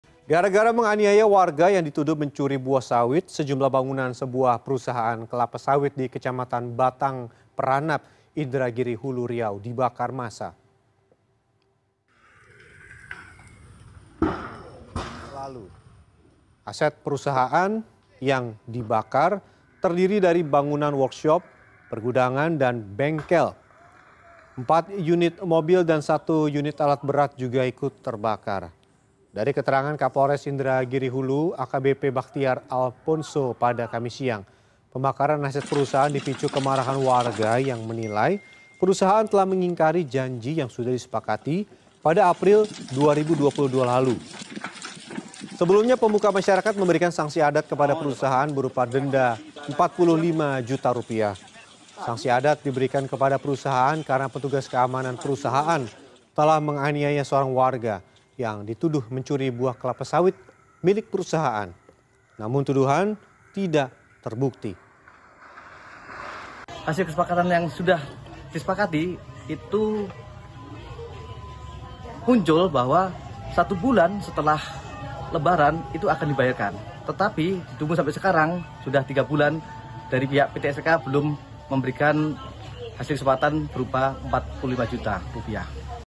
Massa Bakar Bangunan Perusahaan Sawit
Gara gara menganiaya warga yang dituduh mencuri buah sawit. Sejumlah bangunan sebuah perusahaan kelapa sawit di Kecamatan Batang Peranap, Indragiri Hulu, Riau, dibakar massa.